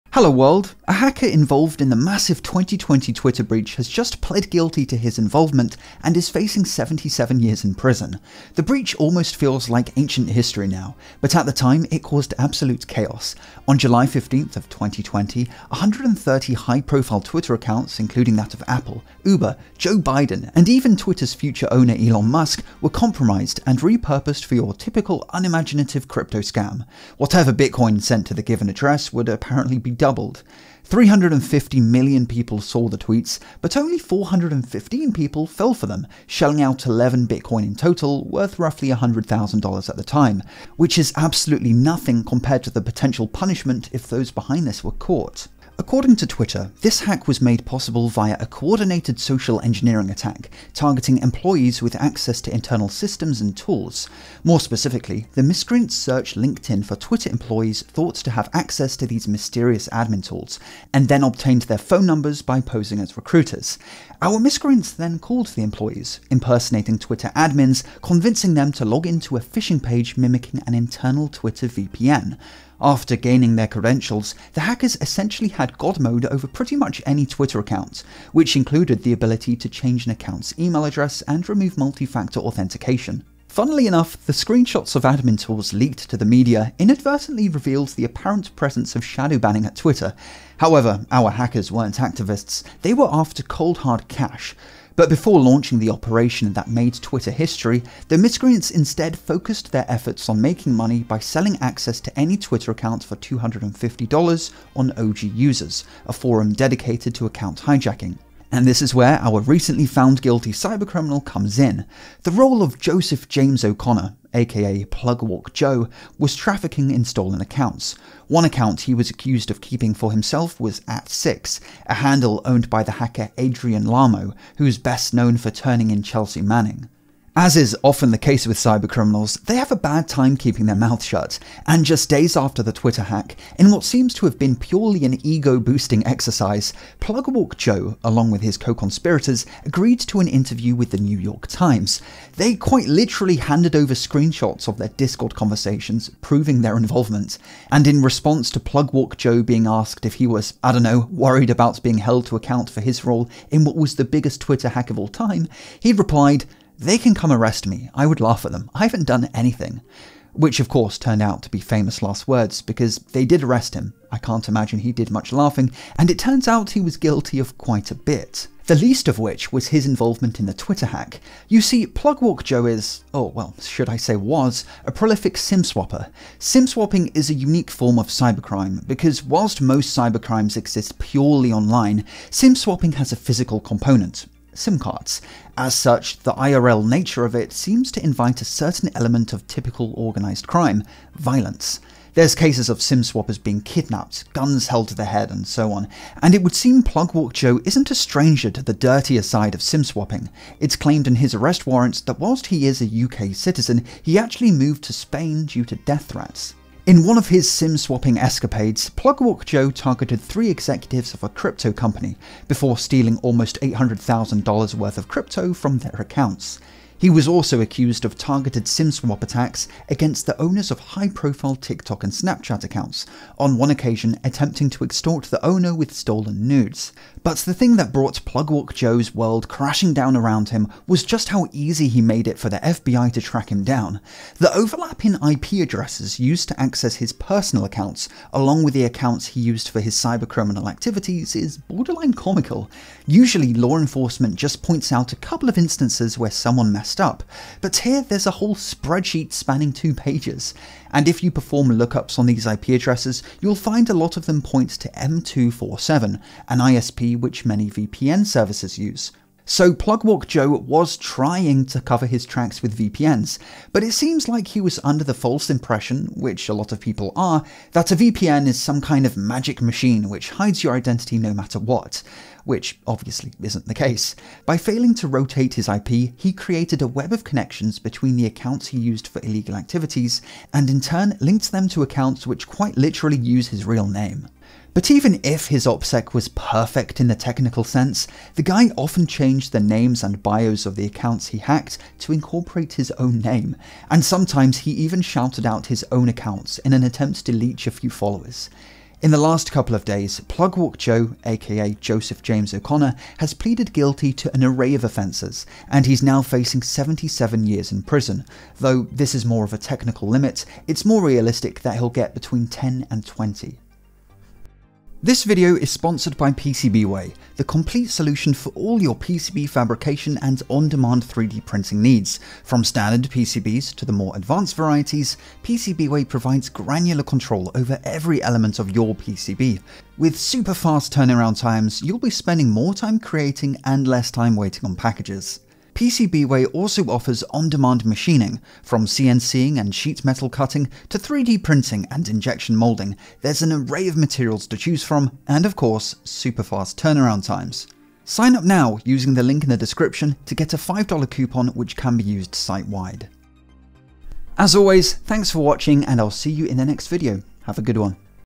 Twitter Hacker Faces 77 Years in Prison
0:00 Overview
0:10 Twitter’s Pwnage
2:06 Keep your mouth shut
2:51 Infamous SIM Swapper
3:45 Endgame
5:14 PCBWay
5:53 Outro